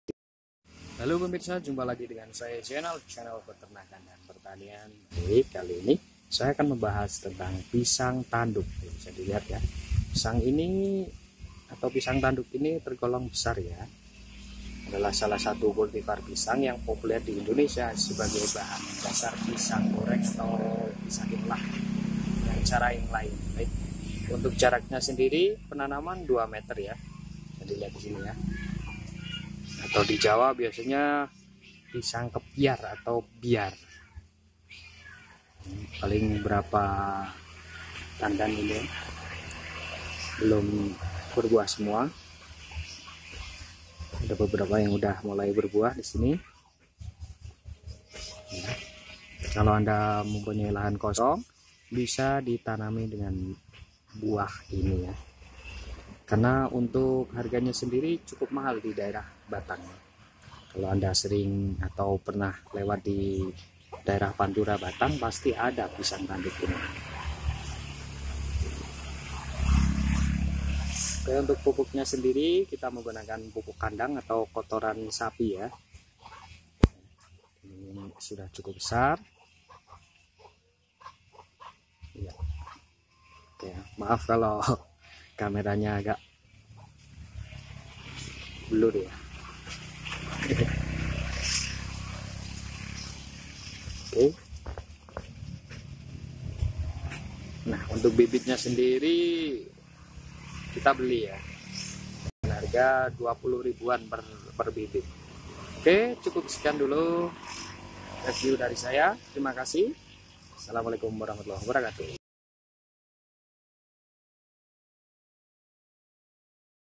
Pisang tanduk ( pyar/kepyar)
Pisang tanduk termasuk pisang yang mudah di olah menjadi berbagai camilan dan mengandung banyak nutrisi yang di butuhkan oleh tubuh kita